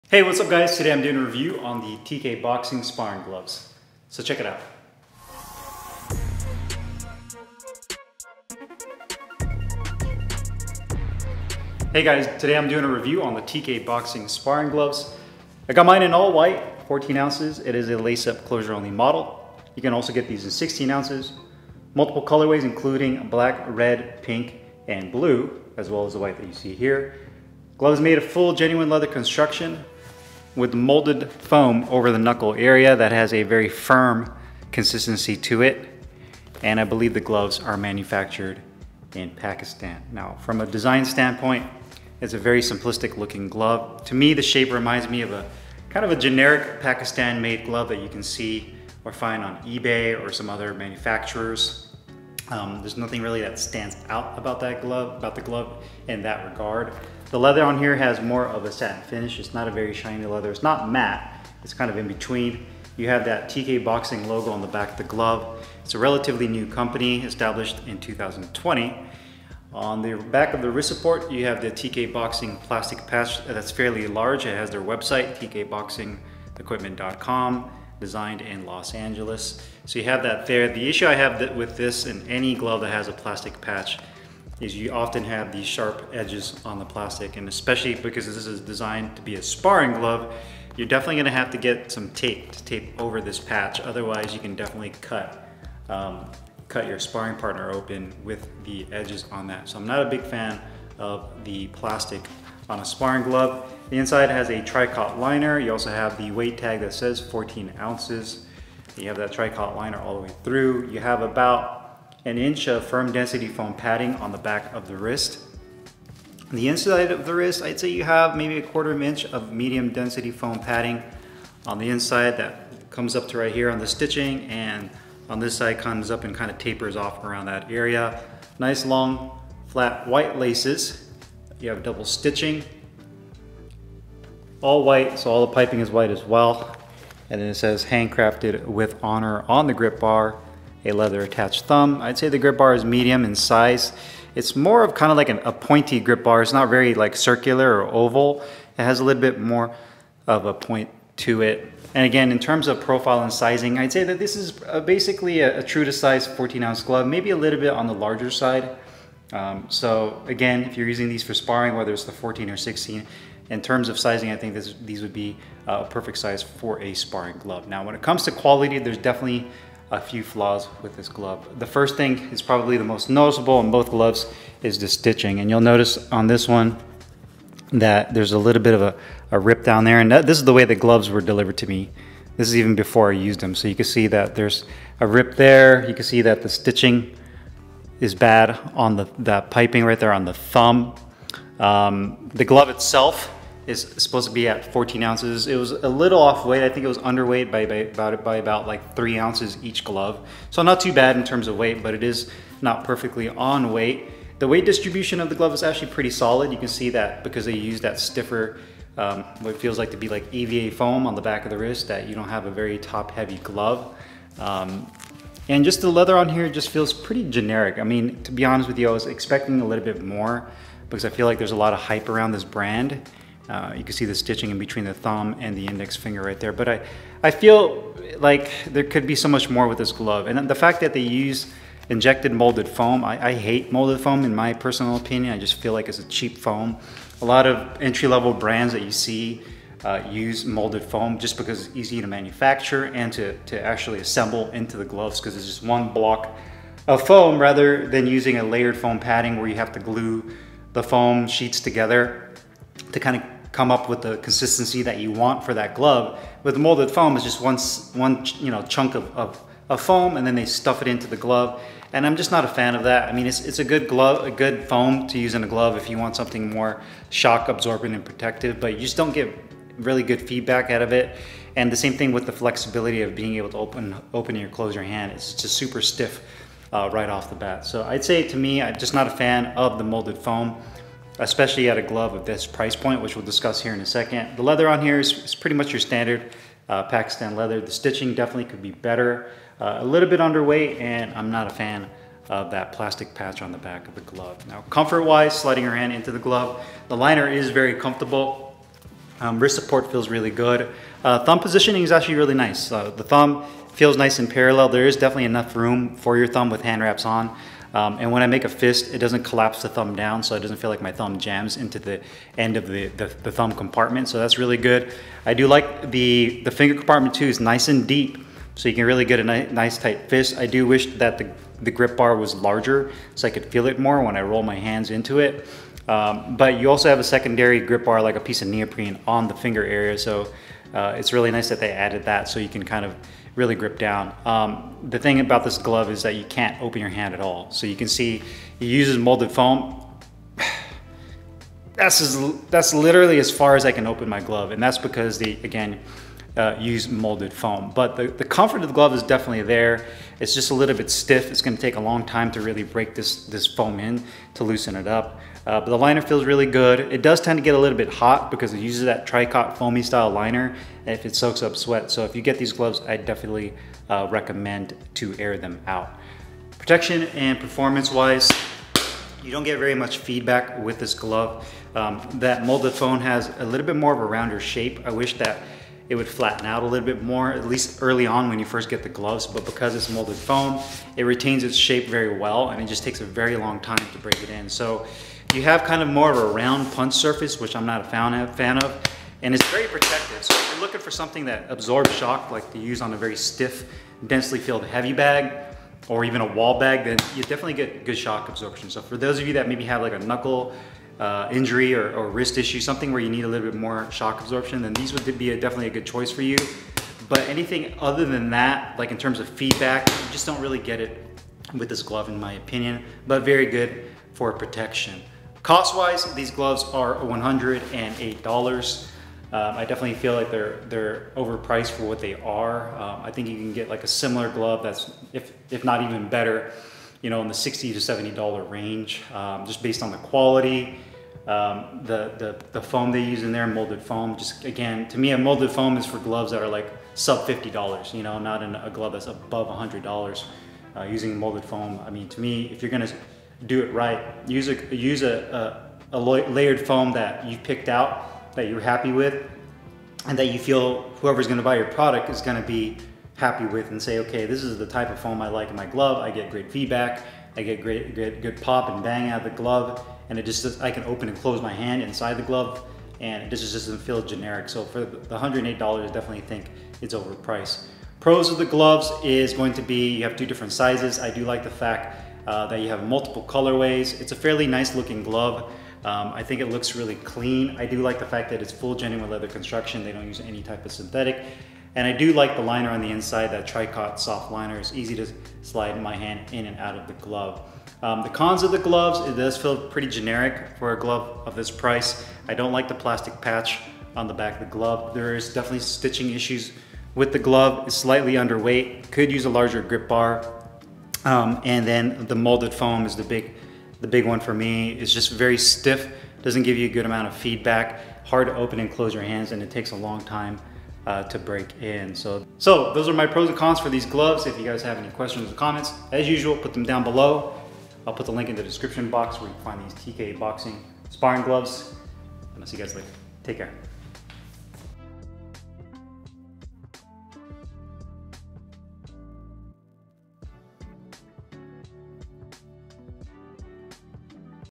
TK Boxing Sparring Gloves REVIEW- AVERAGE GLOVES NEED IMPROVEMENT?!
Timestamps
00:00 - Intro
00:12 - Glove Review
07:49 - Pros/ Cons